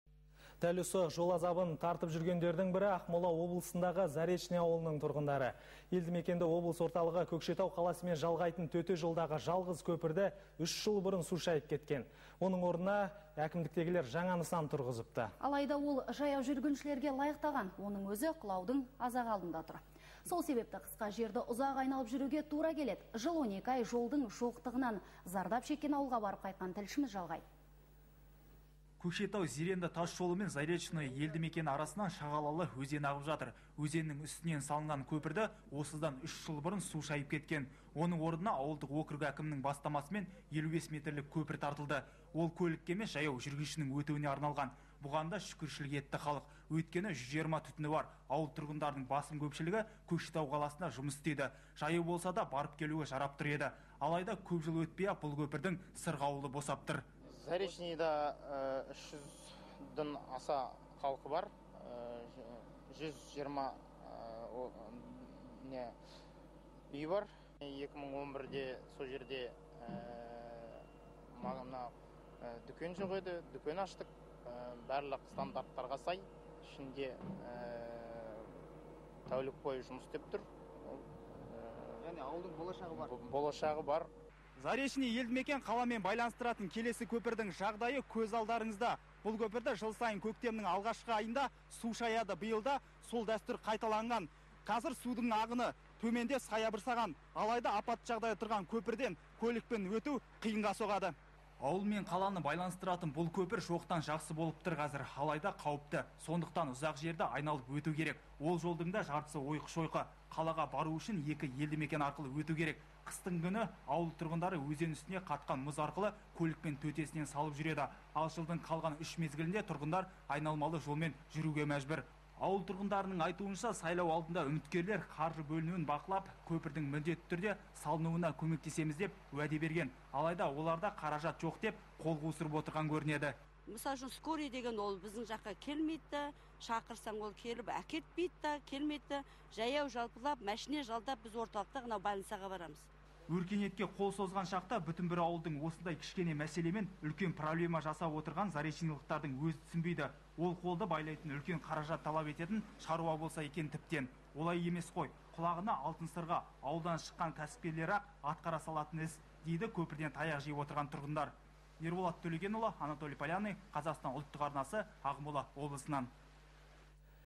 Заречныйлықтар жол азабын тартып жүр
Ақмола облысындағы Заречный аулының тұрғындары көлік көпіріне зар. Елді-мекенді облыс орталығы Көкшетау қаласымен жалғайтын төте жолдағы жалғыз көпірді 3 жыл бұрын су шайып кеткен.